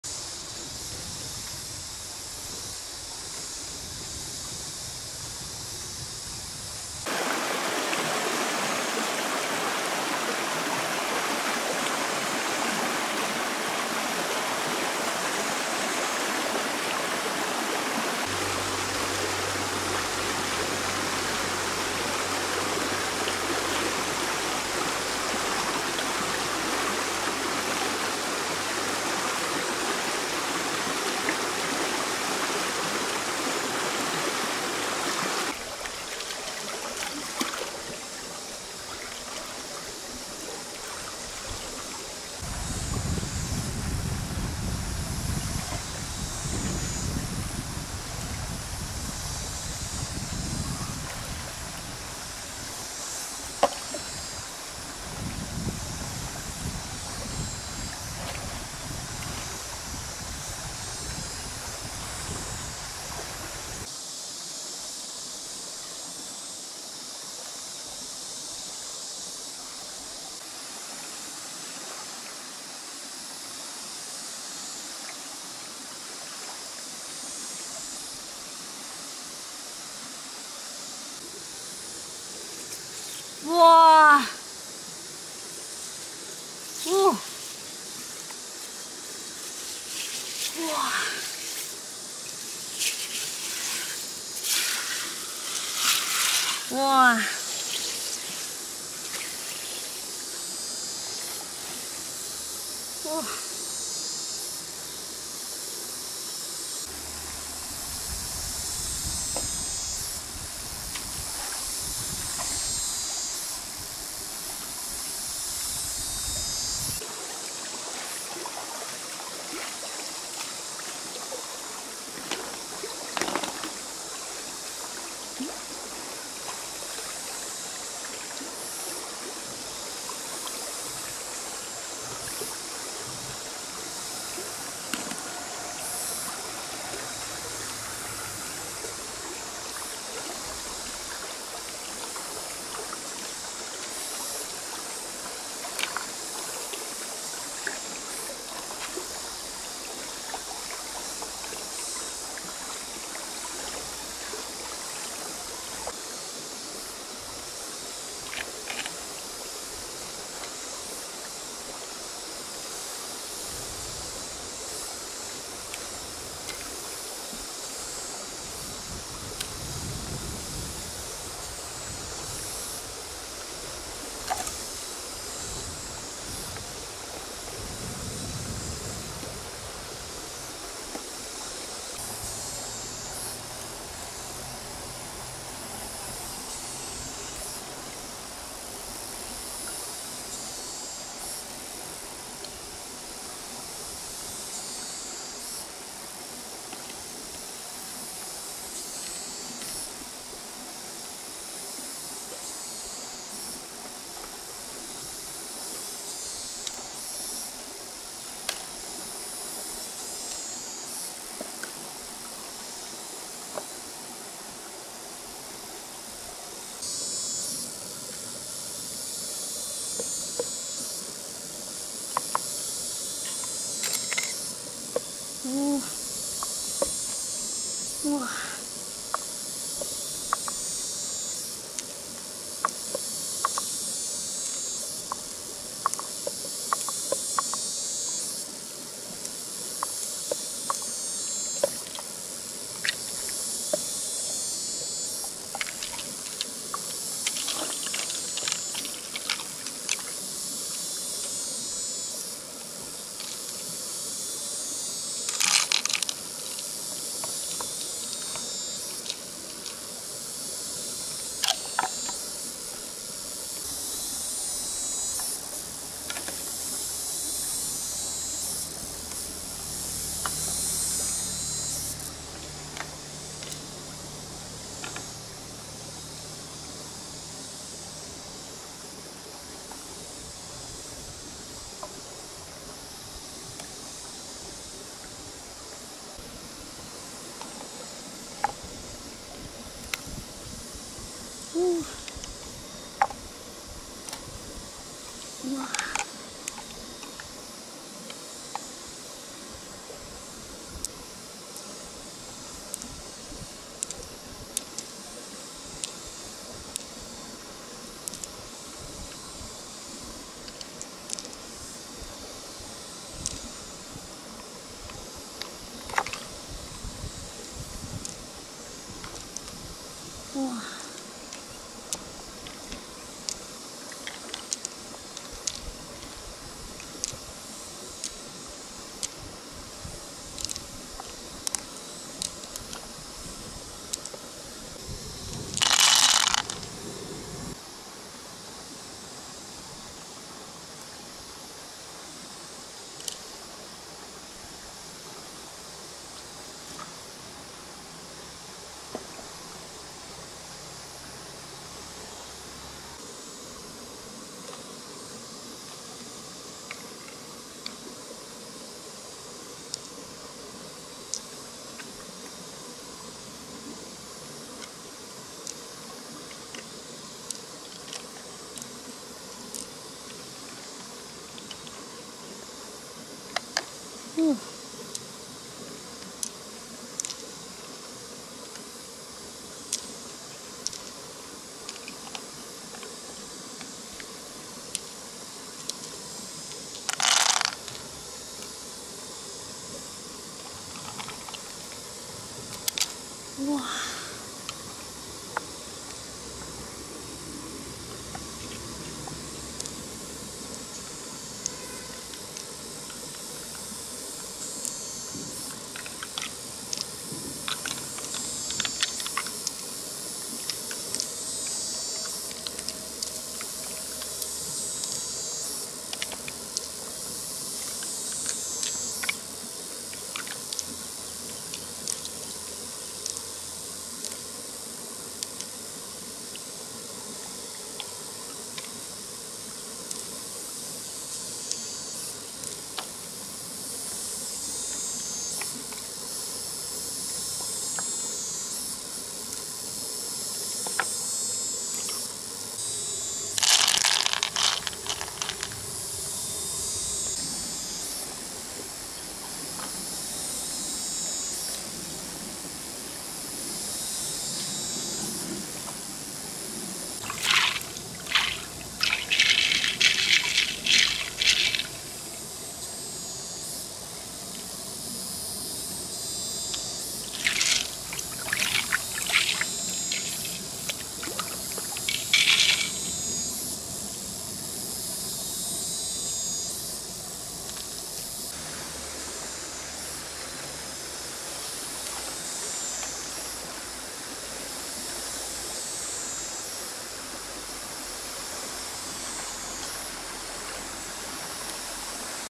The pearl clam hidden in the sand was dug out, bringing me a fortune
Pearlhunter Pearl TreasurehuntI The pearl clam hidden in the sand was dug out, bringing me a fortune